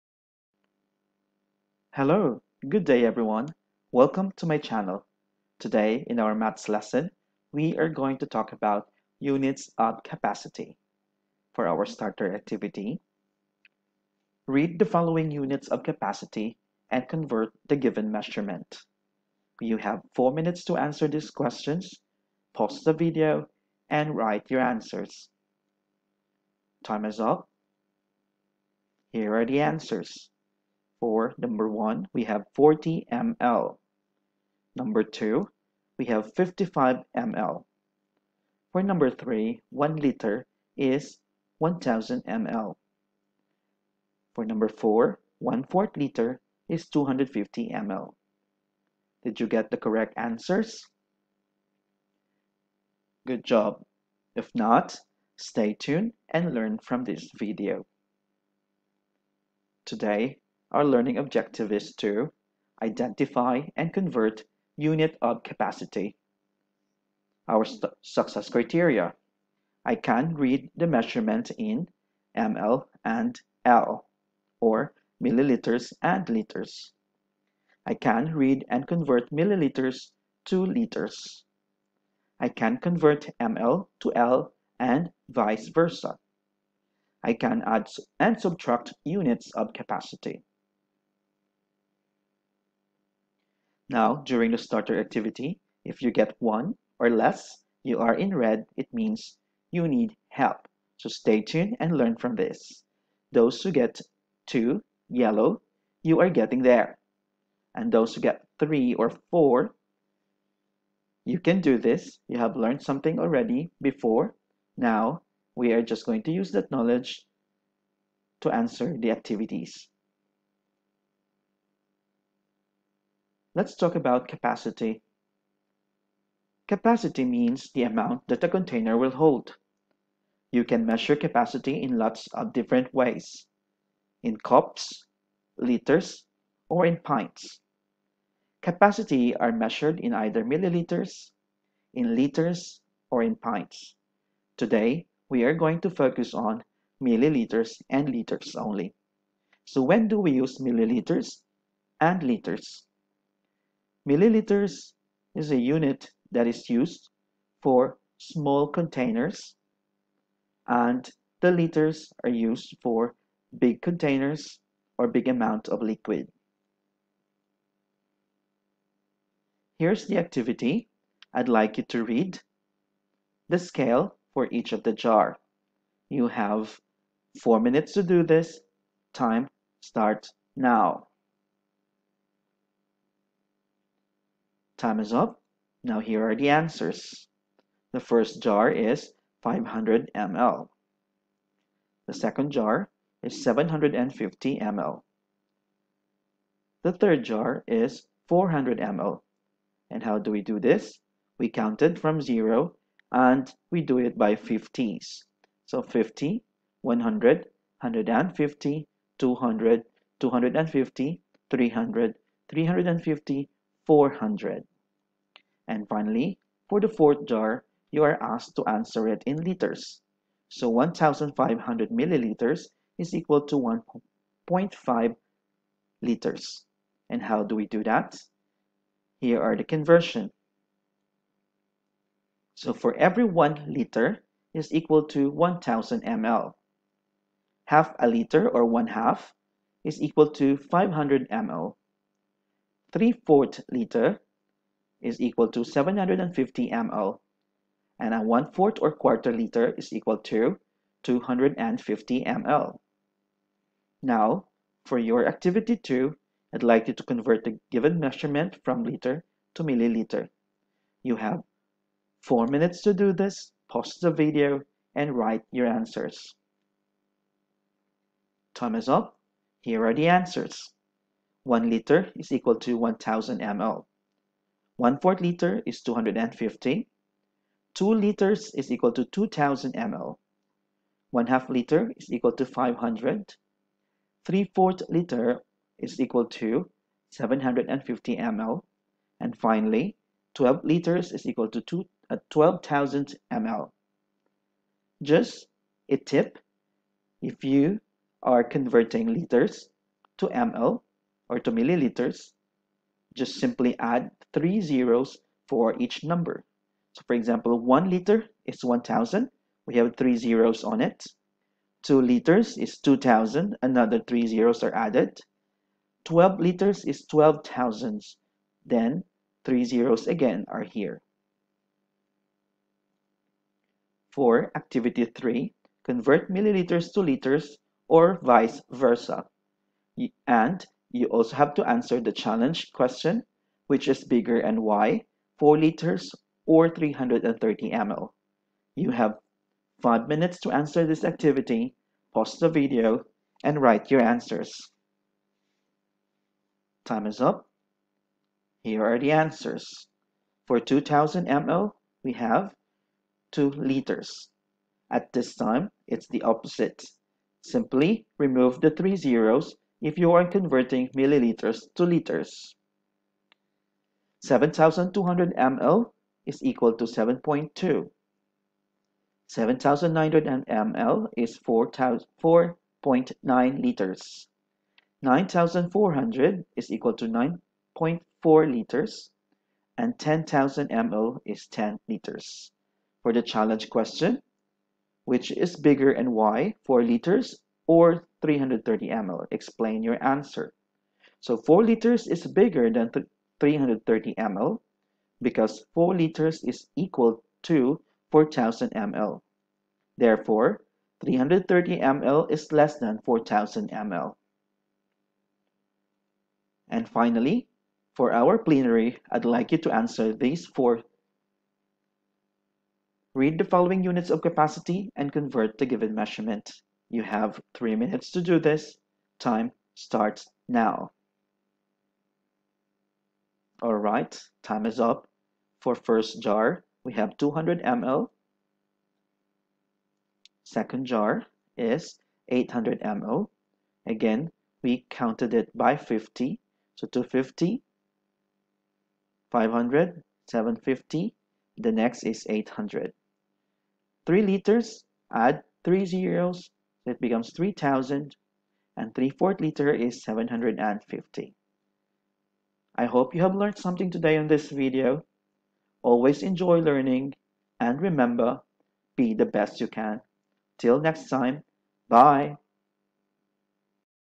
Capacity litres and millilitres
This video lesson is all about Capacity, reading the scale, and converting ml to l or vice versa.

Maths Year 3, NCE
GEMS Winchester School, Abu Dhabi